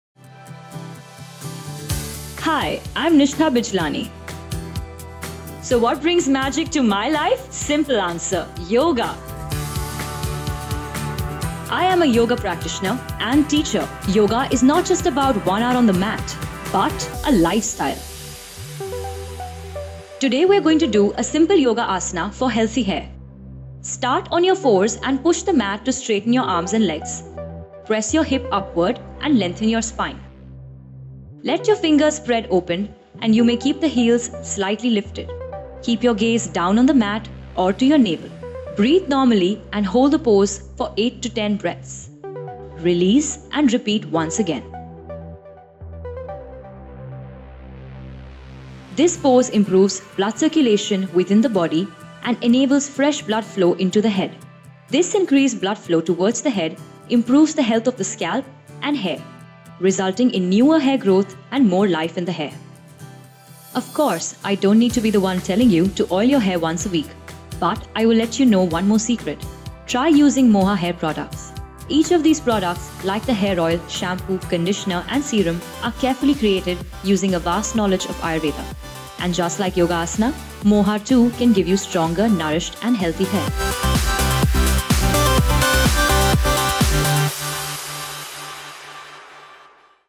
Adho Mukha Svanasana | Hair tips by moha:
Practice Adho Mukha Svanasana commonly known as Downward Facing Dog Pose daily as suggested by yoga-expert @nishthabijlani.
It is an excellent yoga asana which on regular practice helps: •Improves blood circulation to your head.
• Improving hair health.
• Controls hair fall.
Practice Adho Mukha Svanasana daily 8-10 times morning or evening, as time permits, followed by weekly hair massage with moha: 5-in-1 Hair Oil for hair-fall control.

And it’s much suggested to introduce yoga to our life which is just not spending 1-hour on mat but is a way to life which will help deal with daily stress. .